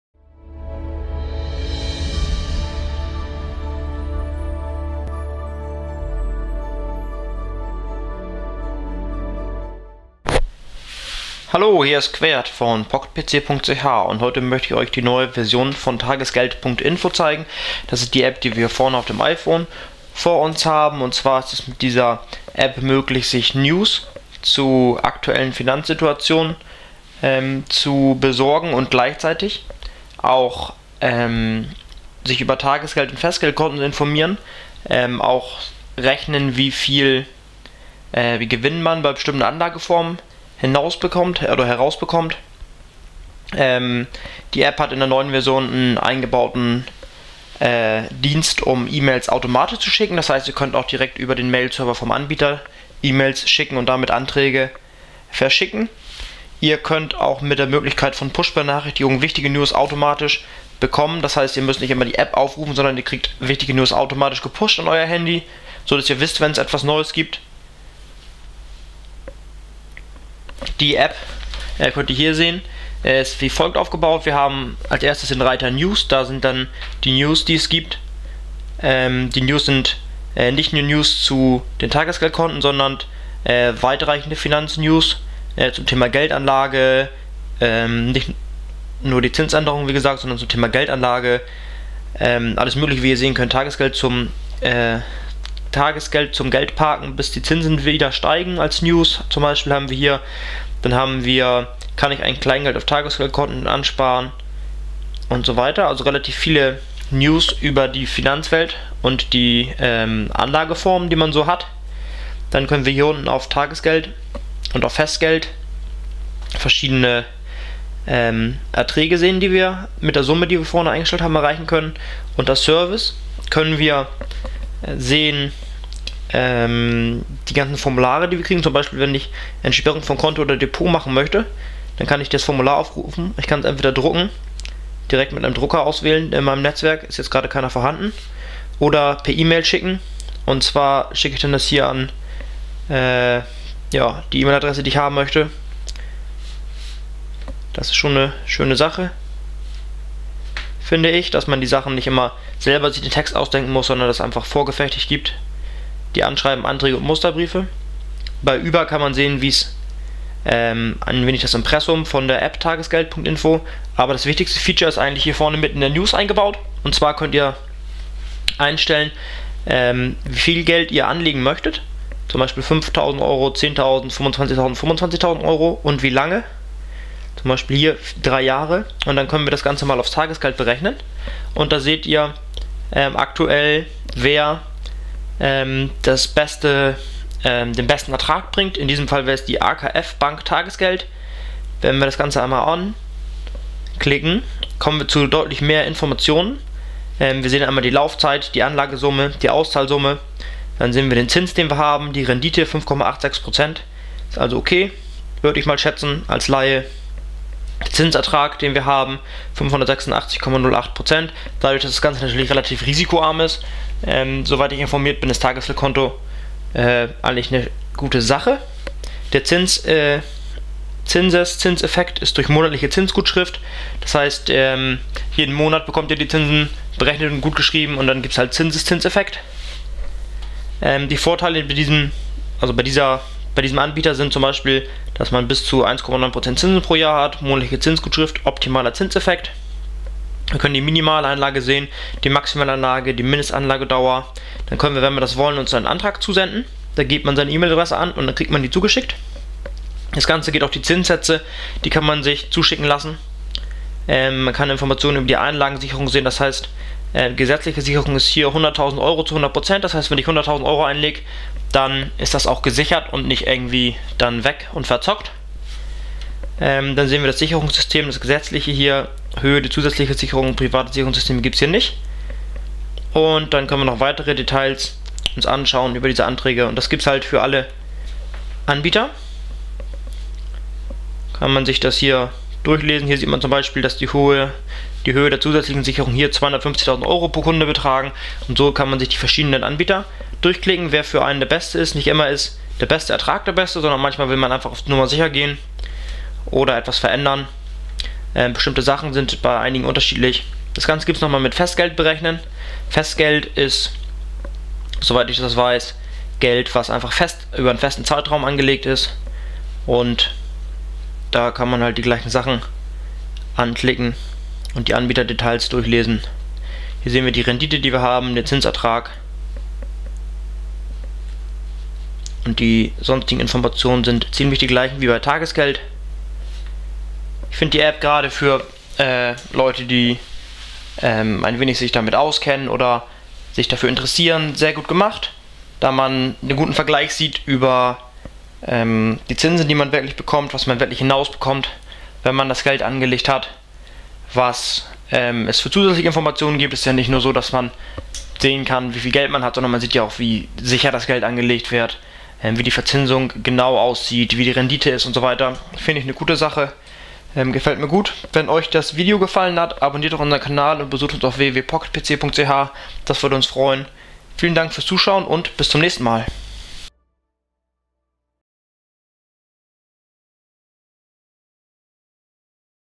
Tagesgeld.info für das iPhone vorgestellt
zeigt euch in diesem Video die kostenlsoe App Tagesgeld.info für das iPhone